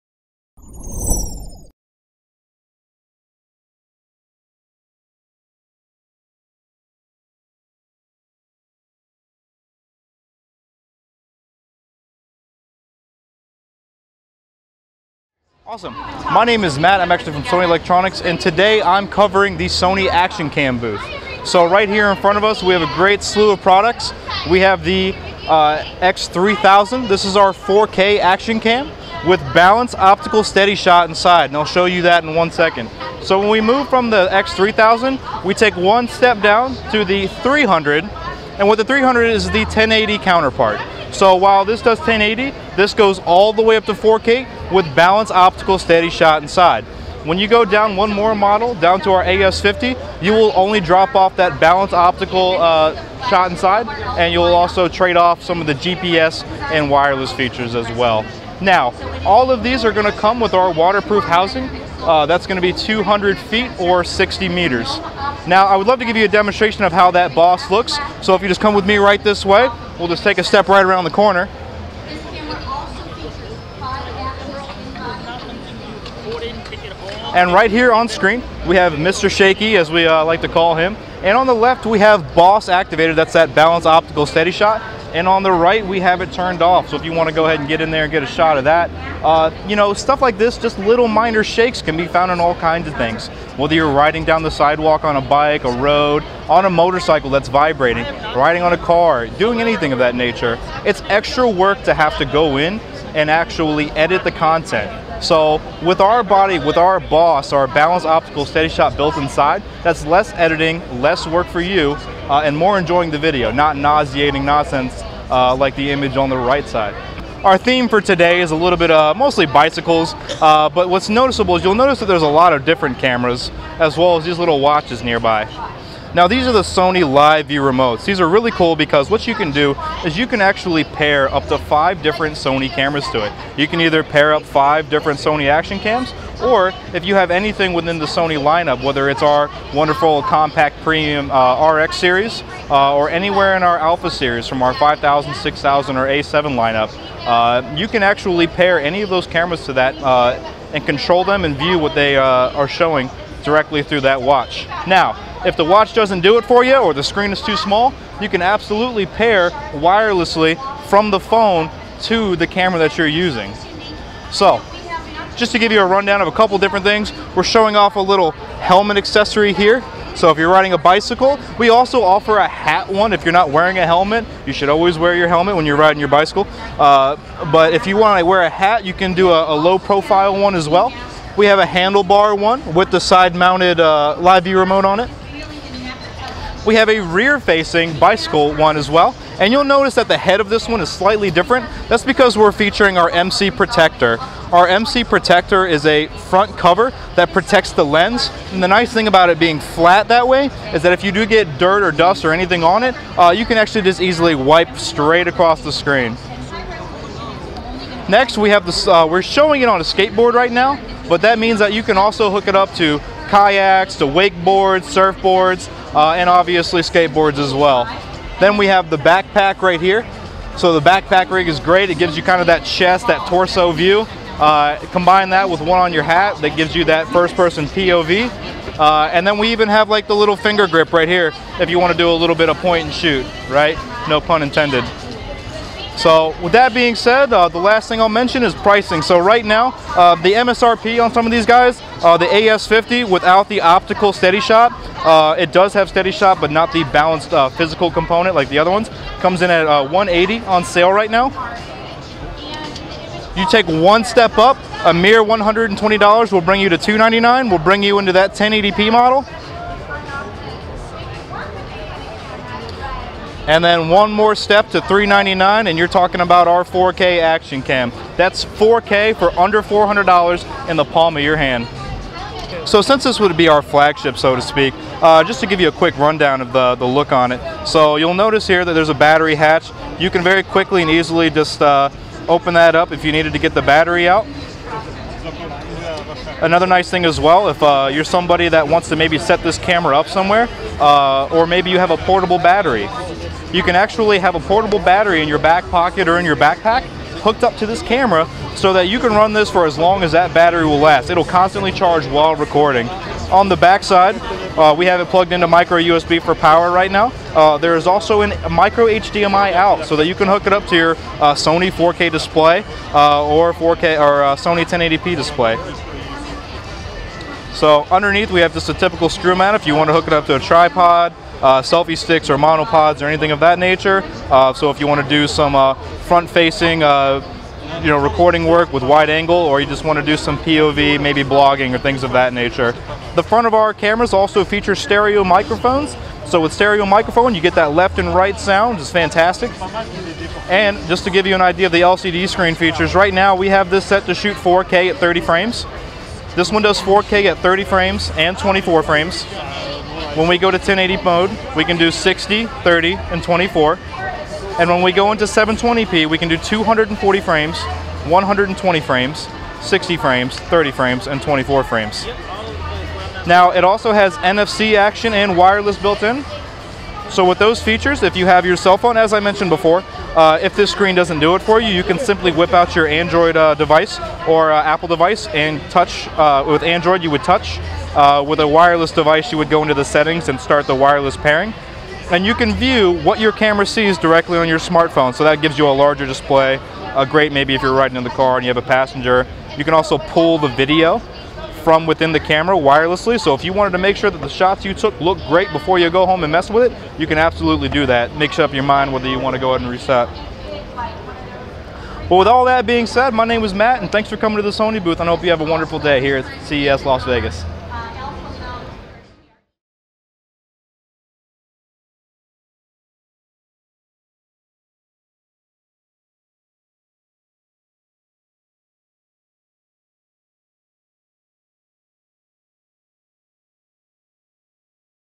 CES 2017 | Sony Action Cam Full Lineup | FDR-X3000R 4K Camera | SmartReview.com
SmartReview.com attended CES 2017 to see the latest in Camera technology.  We got a total rundown on all the latest Sony Action Cams available.  The FDR-AS3000R is the flagship of the line with Balance Optical Steadyshot image stabilization and 4K video capture.